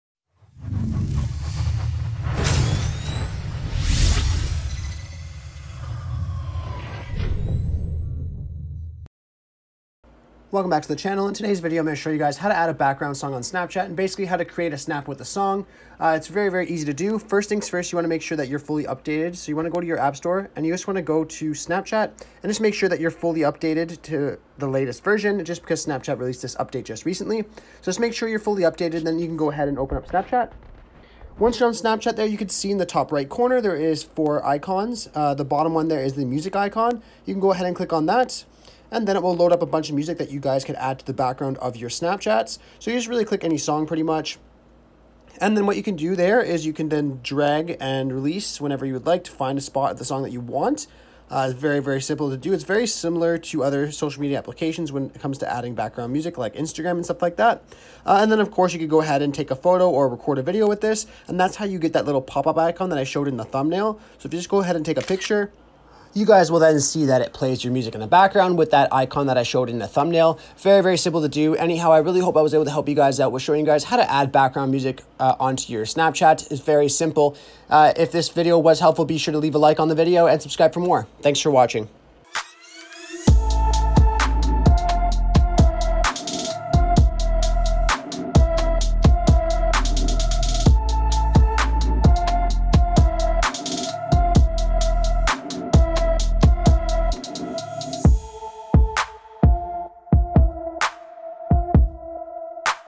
How To Add Background Song On Snapchat 👻| Create Snap With A Song | Snapchat Music Update
In today's video we show you How To Add Background Song On Snapchat.

Click Here For More Snapchat Tutorials/Tricks!